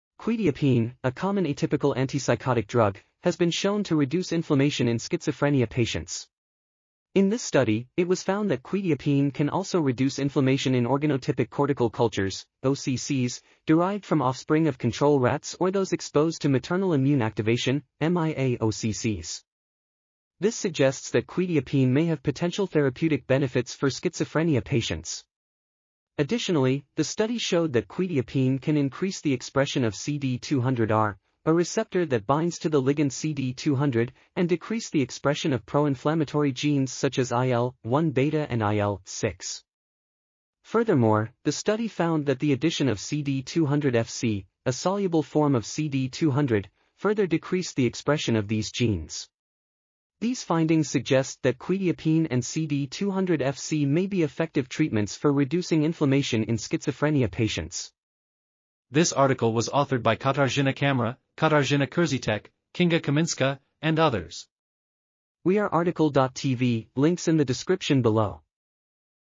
Insights into the Potential Impact of Quetiapine on the Microglial Trajectory and Inf... | RTCL.TV
### Keywords ###

### Article Attribution ###
Title: Insights into the Potential Impact of Quetiapine on the Microglial Trajectory and Inflammatory Response in Organotypic Cortical Cultures Derived from Rat Offspring
Authors: Katarzyna Chamera, Katarzyna Curzytek, Kinga Kamińska, Ewa Trojan, Monika Leśkiewicz, Kinga Tylek, Magdalena Regulska ,and Agnieszka Basta-Kaim

### Video Timestamps ###
0:00:00 - Summary
0:00:58 - Title
0:01:05 - Outro
0:01:09 - End